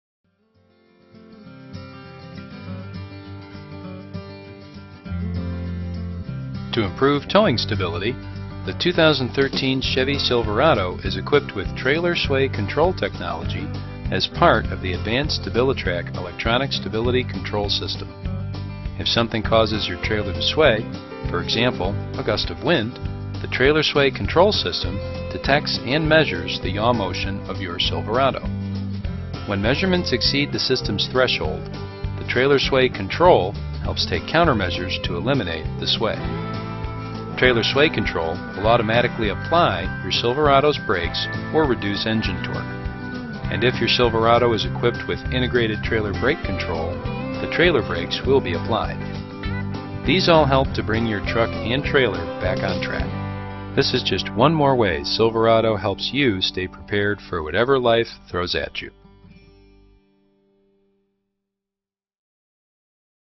2013 Silverado Trailer Sway Control
Trailer Sway Control Standard on all Chevrolet Silverados

The 2013 Chevrolet Silverado available at Crotty Chevrolet Buick is now better equipped to help prevent drivers and their trailers from uncontrolled trailer swinging with the advanced StabiliTrak® electronic stability control system with integrated trailer sway control.
Trailer sway control employs an angle sensor within the steering wheel assembly to measure the drivers intended yaw, or rotational movement around the axis of a vehicle, which determines the direction. During a sway incident, the sensor detects the driver's intended yaw, while another sensor measures the actual yaw. The Electronic Brake Control Module compares both inputs and sends a signal to the Integrated Trailer Brake Control for action. The resulting action is to apply the trailer brakes and reduce the velocity. In more extreme conditions, the Integrated Trailer Brake Control will also signal the vehicle to apply its brakes until the oscillation stops.
"Silverado owners can rest assured that their trucks were built with their safety in mind and can tow with extra confidence thanks to trailer sway control which is always at the ready," said John Crotty, owner of Crotty Chevrolet Buick in Corry, PA. "Trailer sway control is just one more way Silverado helps you stay prepared for whatever life throws at you."